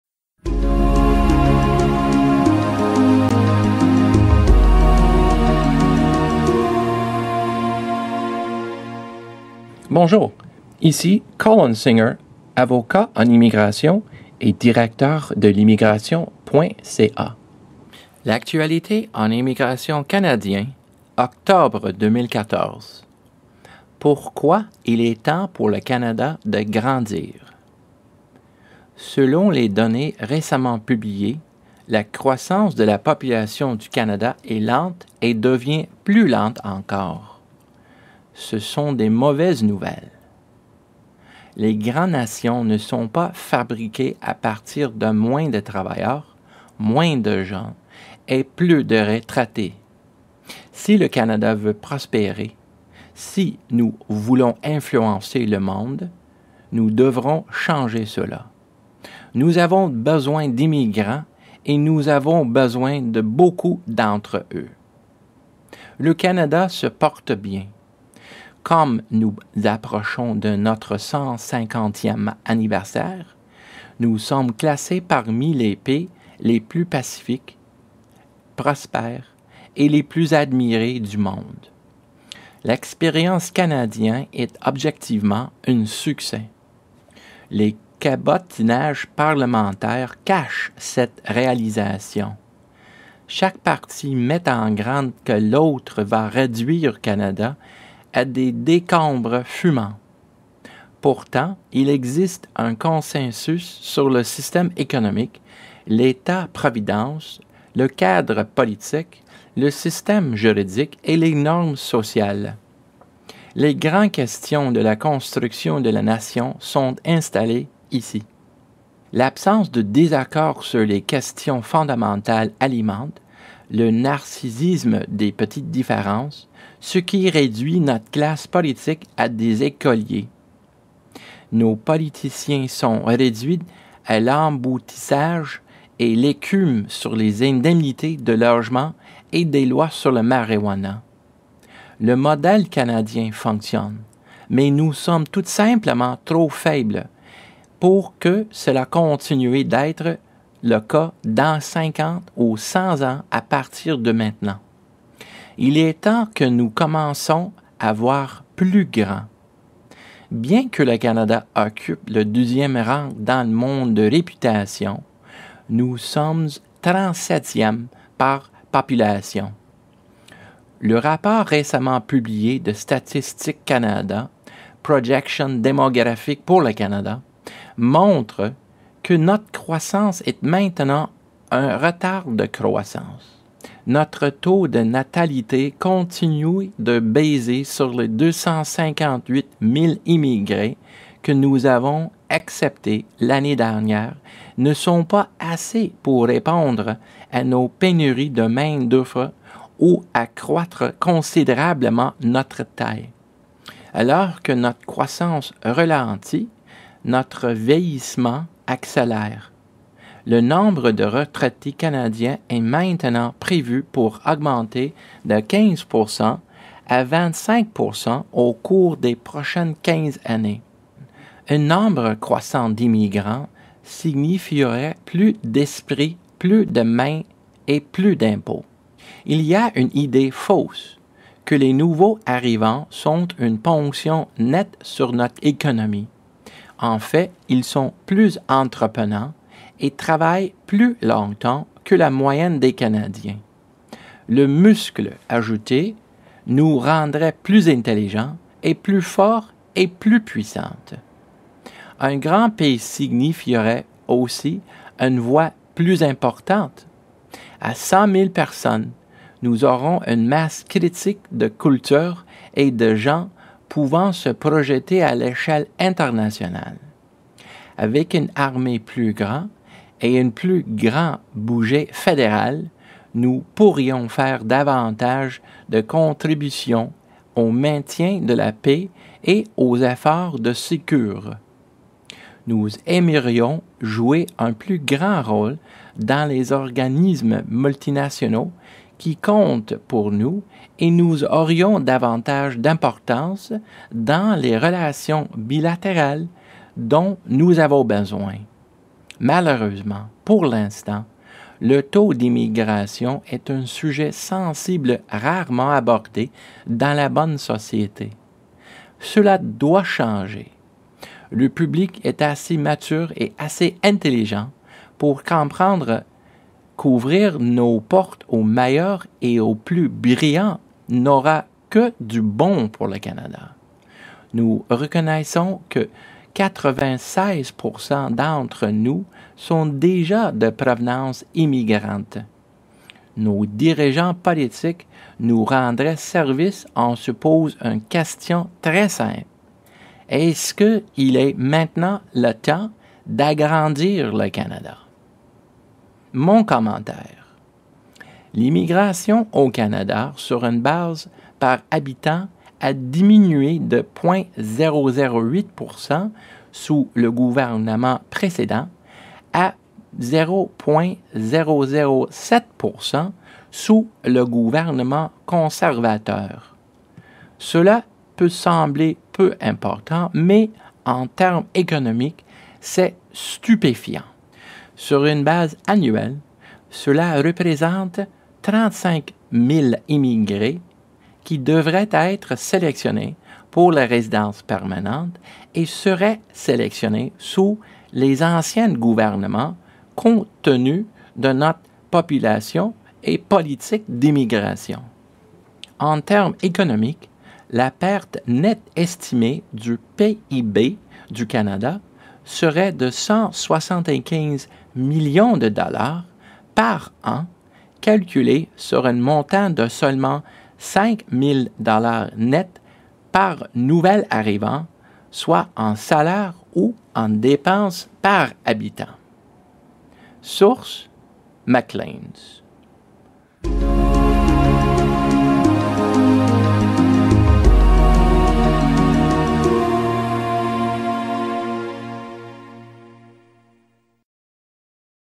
Pourquoi il est temps pour le Canada de grandir
Selon les données récemment publiées, la croissance de la population du Canada est lente et devient plus lente encore. Ce sont de mauvaises nouvelles. Les grandes nations ne sont pas fabriquées à partir de moins de travailleurs, moins de jeunes et plus de retraités. Si le Canada veut prospérer, si nous voulons influencer le monde, nous devrons changer cela. Nous avons besoin d'immigrants et nous avons besoin de beaucoup d'entre eux.